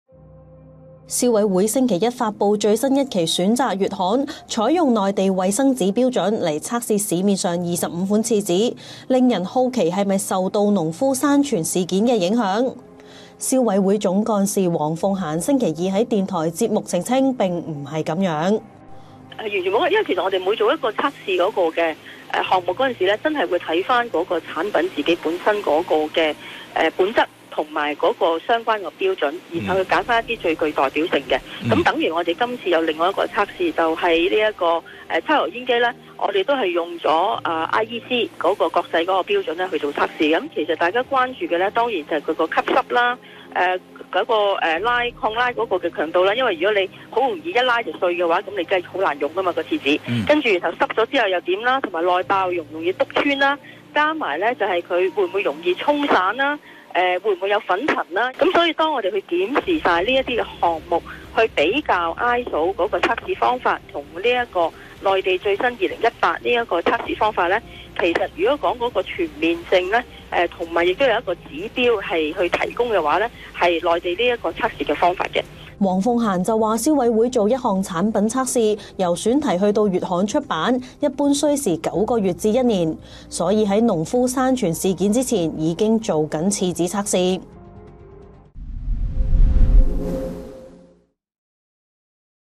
消委會廁紙︱測試採內地標準 黃鳳嫺否認受農夫山泉事件影響 「完全無關，純粹按產品而定」｜星島頭條新聞｜廁紙｜國際標準｜內地標準｜消委會｜農夫山泉｜黃鳳嫺
消委會上月發布新一期選擇月刊時，測試項目之一的「農夫山泉飲用天然水」溴酸鹽含量達歐盟上限，「農夫山泉」發律師信指誤導、要求道歉，最終消委會「跪低」，將其獨立分類為「飲用天然水」並將其整體表現由4.5星調整至5星。消委會昨日（19日）發布新一期選擇月刊，當中測試和比較25款廁所卷紙，採用的是內地衞生紙標準，而非過往常用的國際或歐盟標準，引起好奇是否受「農夫山泉」事件影響。消委會總幹事黃鳳嫺今早在商台節目表示，完全沒有關係。